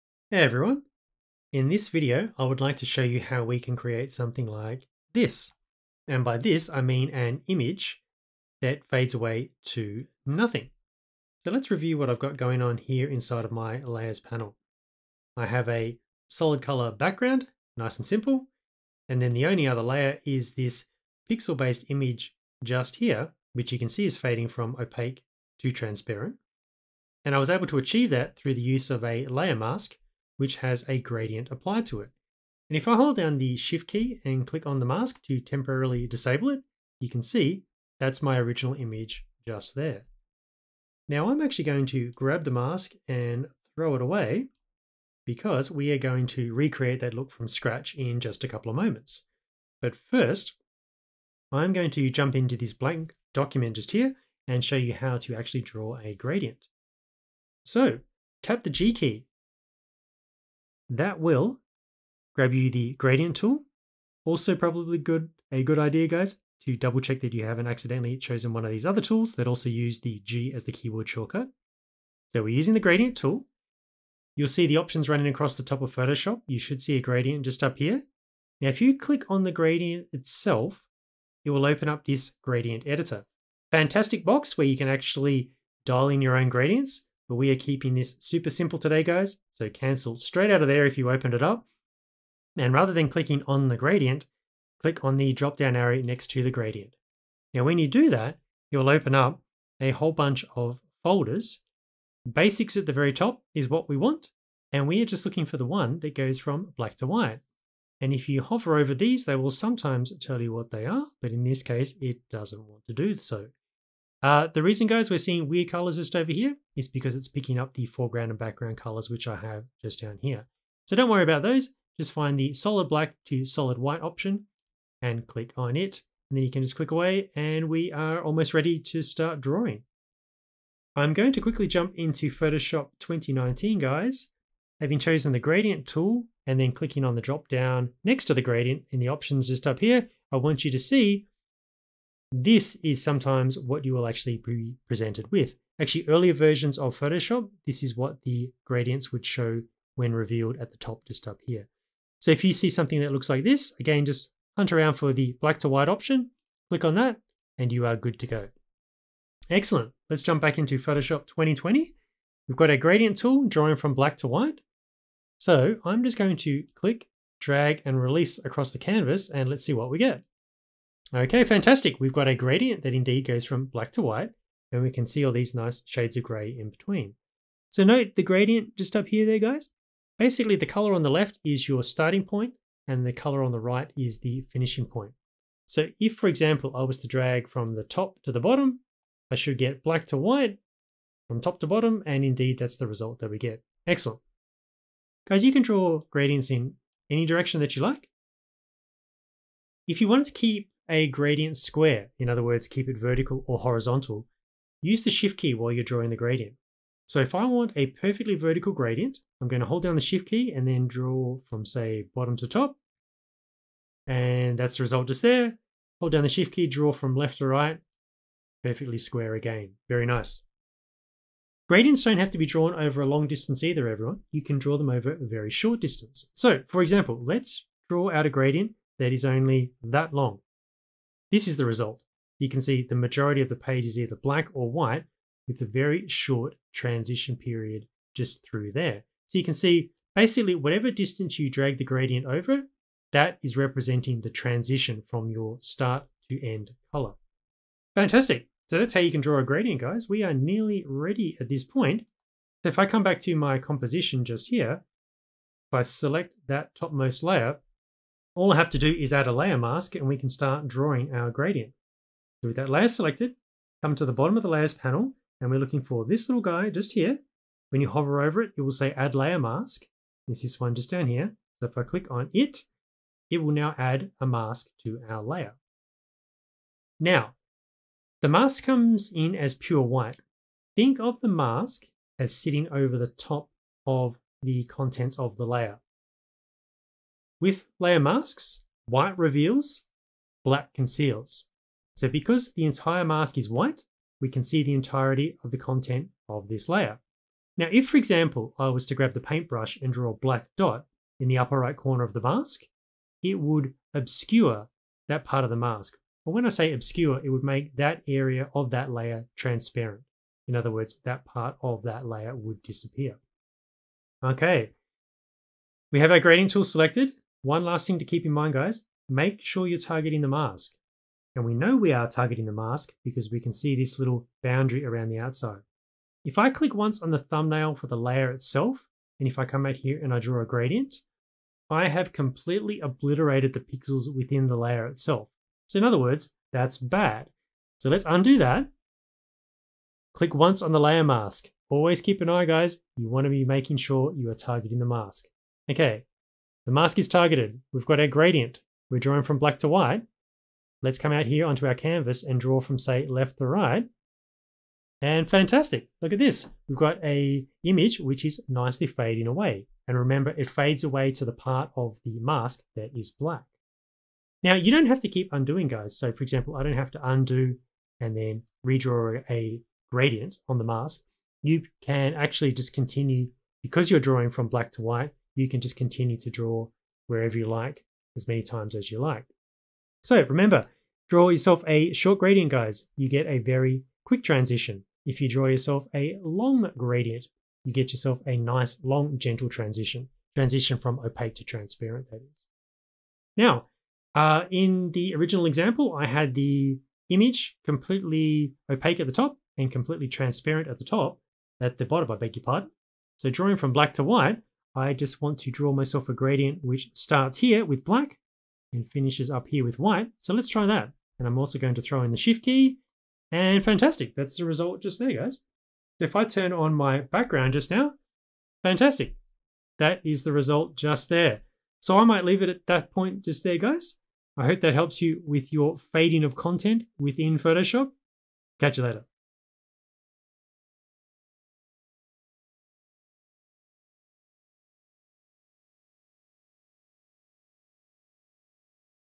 Fade an Image to Transparent in Photoshop
Learn how to fade an image to transparent in Photoshop with a gradient applied to a mask. Details below...

0:00 - Introduction

0:49 - Drawing a Gradient
Having chosen the Gradient Tool (keyboard shortcut: G), gradient options will be shown across the top of Photoshop. Clicking on the gradient will open up the Gradient Editor where any gradient can be manually created. Clicking on the drop down arrow next to the gradient allows for the selecting of pre-made gradients. This tutorial makes use of the Black to White gradient.

To draw a gradient, click, drag and release the mouse anywhere across the Photoshop canvas.

Make a gradient square (horizontal or vertical) by holding down the Shift key while drawing the gradient.

4:12 - Adding a Mask to a Layer
With a layer selected, click the "Add a mask" button at the bottom of the Layers panel.

When working with masks, "white reveals, black conceals". White parts of a mask will reveal the equivalent portions of a layer while the black parts of a mask will conceal (hide) the equivalent portions of a layer. Shades of grey partially hide the equivalent layer content.

5:15 - Applying a Gradient to a Mask
Before drawing a gradient, ensure that the mask, not the layer thumbnail, is selected. This prevents drawing over the pixels on that layer.

A short gradient drawn on a layer mask will create a quick transition between the opaque and transparent parts of a layer.

A long gradient drawn on a layer mask will create a slow transition between the opaque and transparent parts of a layer.

Thank you to Paul Gilmore